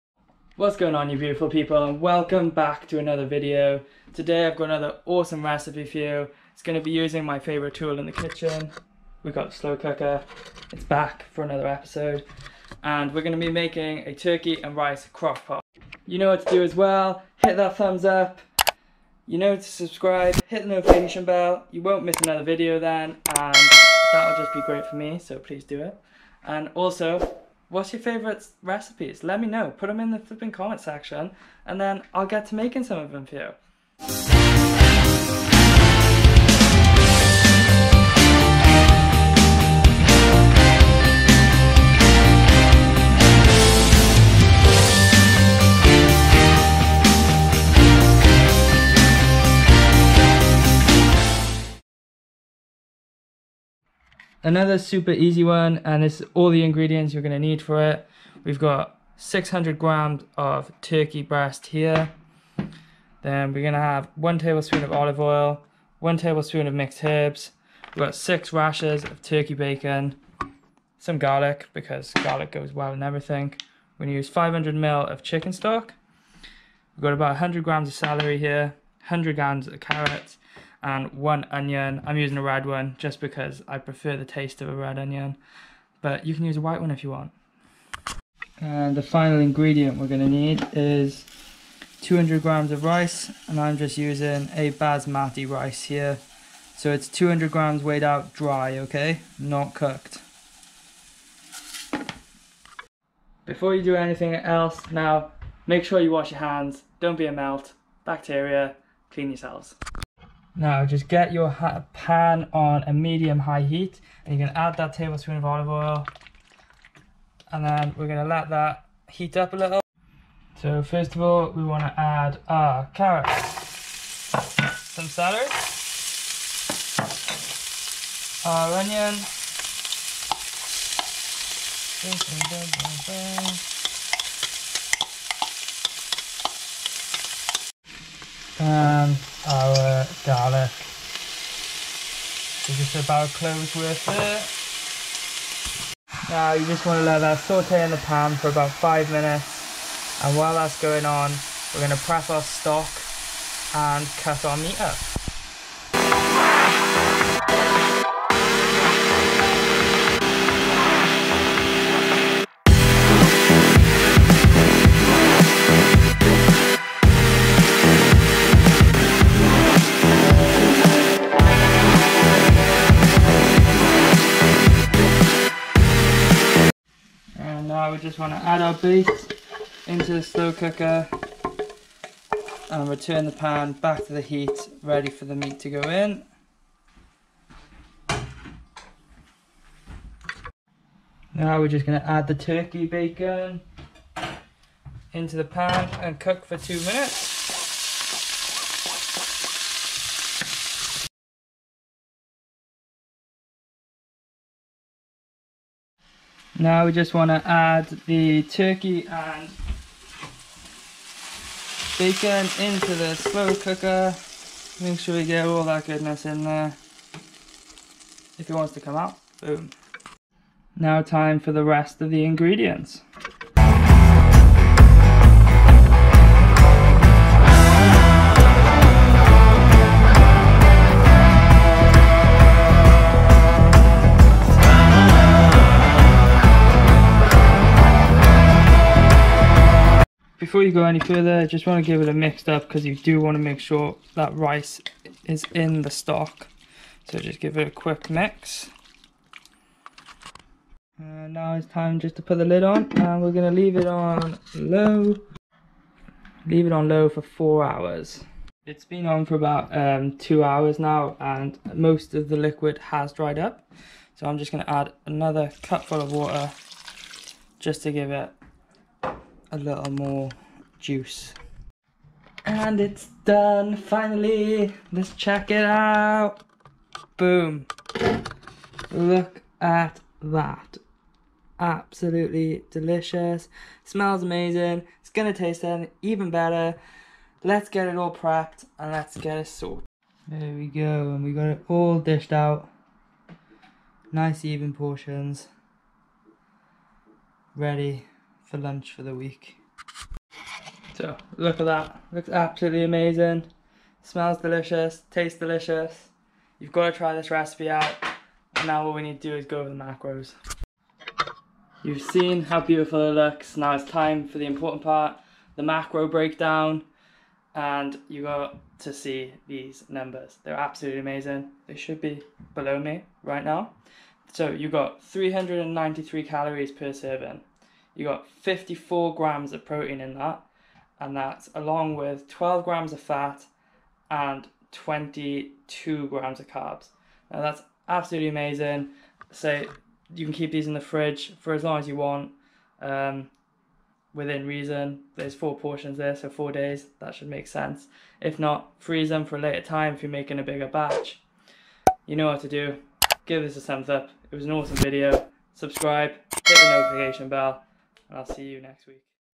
Slowcooker One-pot Turkey and Rice! High Protein Meal Prep! (Ep.6)
Welcome to the champions lifestyle. It’s episode 6 champions! This episode will be looking at a super tasty slow cooker recipe. A one-pot turkey and rice healthy crockpot recipe that has a massive 54g protein per serving. It's a super easy recipe that will help you achieve your goals be that muscle gain or weight loss. Also a beginner friendly recipe.

Ingredients:
600g Turkey Breast
6 slices Turkey Bacon
100g Celery
100g Carrots
1 Onion
1 Garlic Clove
500ml Chicken Stock
200g Rice (use what you like I used basmati)
1 tbsp Olive Oil
1 tbsp Mixed Herbs

Macros: 393 Cals / 54P / 22C / 12F